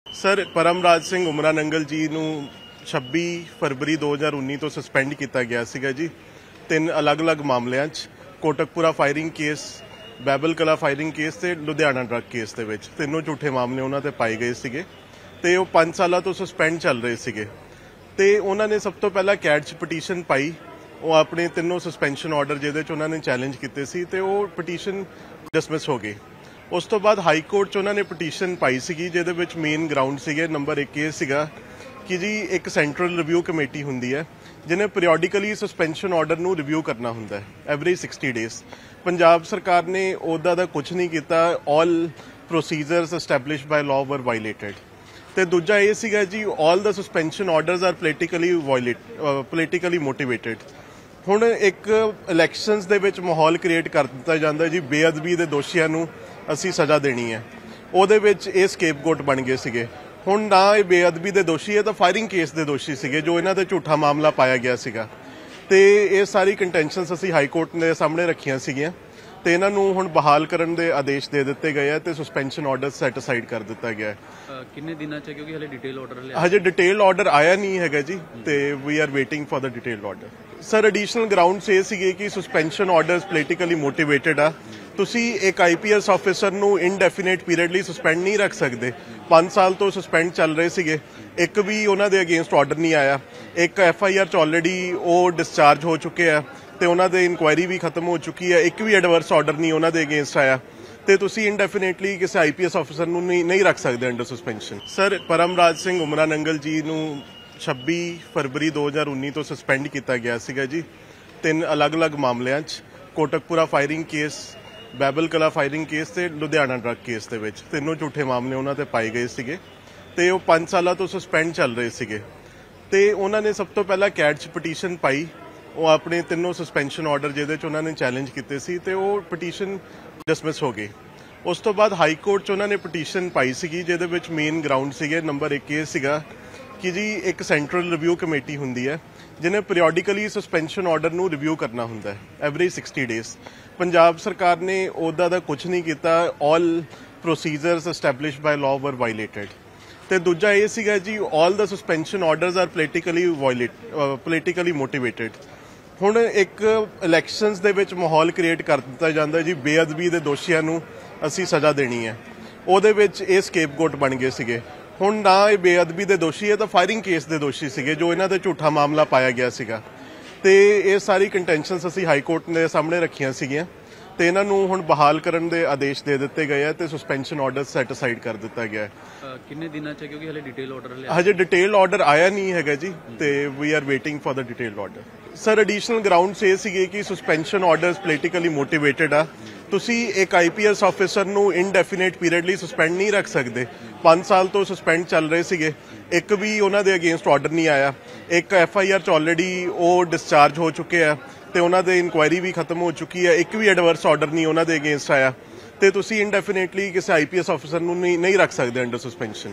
Kotkapura and Behbal Kalan firing case | ਸਸਪੈਂਡ IG ਕਿਵੇਂ ਹੋਇਆ ਬਹਾਲ? ਵਕੀਲ ਨੇ ਦੱਸੀ ਵਿਚਲੀ ਗੱਲ | N18V
ਸਸਪੈਂਡ IG ਉਮਰਾਨੰਗਲ ਕਿਵੇਂ ਹੋਇਆ ਬਹਾਲ? ਵਕੀਲ ਨੇ ਦੱਸੀ ਵਿਚਲੀ ਗੱਲ, ਕੋਟਕਪੂਰਾ ਤੇ ਬਹਿਬਲ ਕਲਾਂ ਫਾਇਰਿੰਗ ਮਾਮਲੇ  'ਚ ਦਰਜ ਸੀ FIR